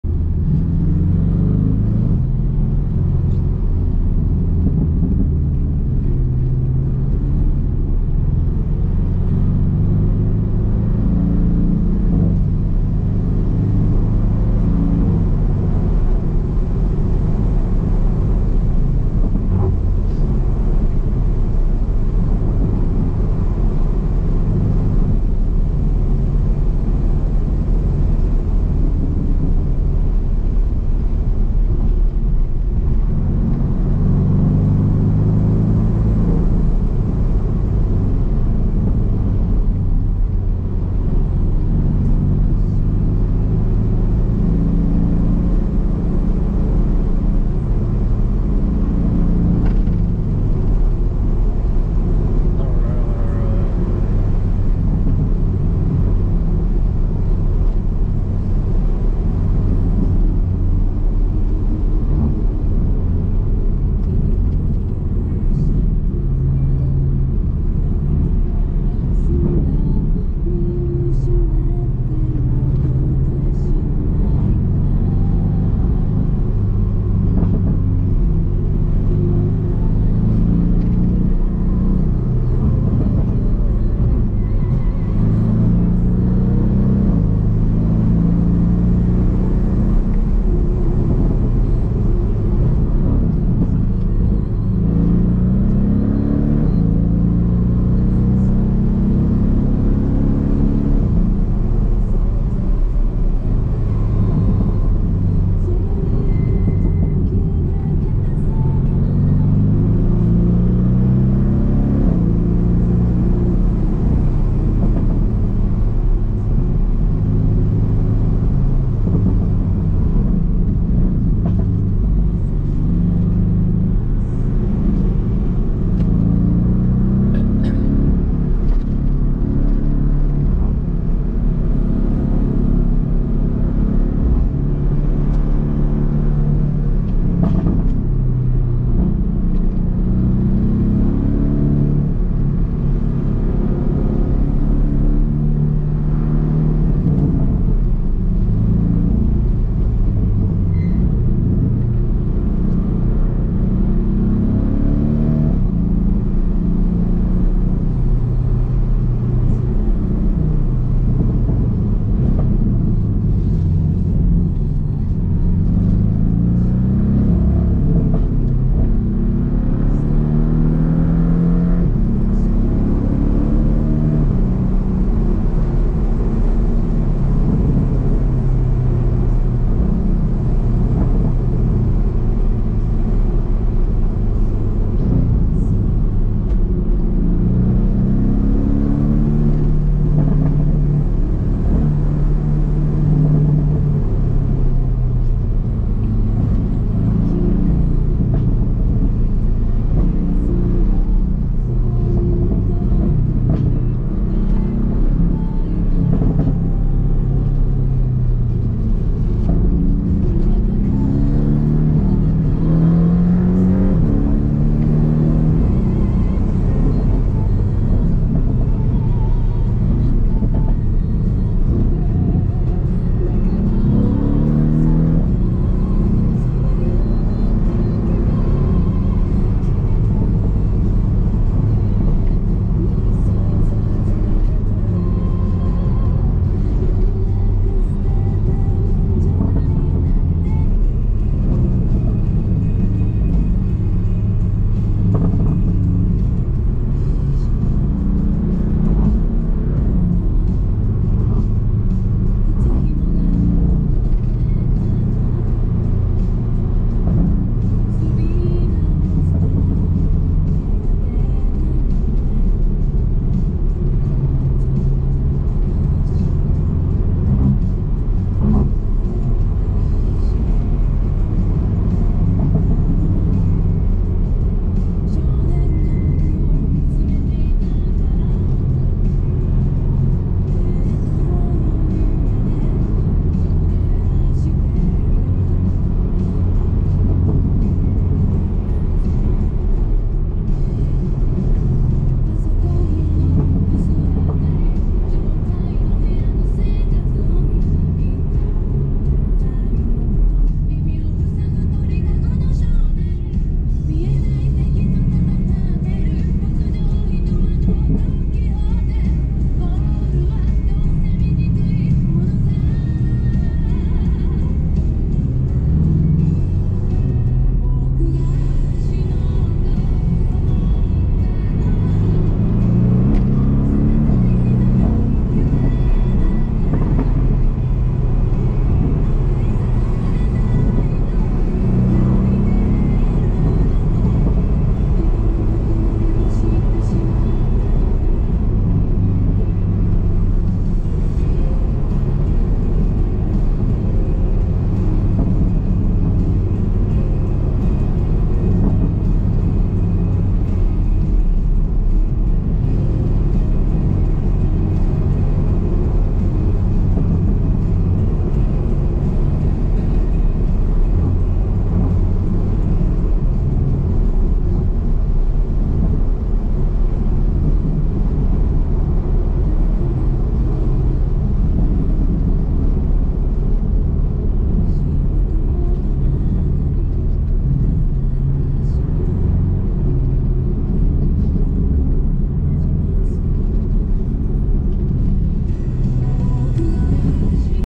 澳底Rtis gtsilver #982 cayman |Endless HC+ 0210
等順哥來會合的時間,
先來102甲走一走,
這台好像還沒來過。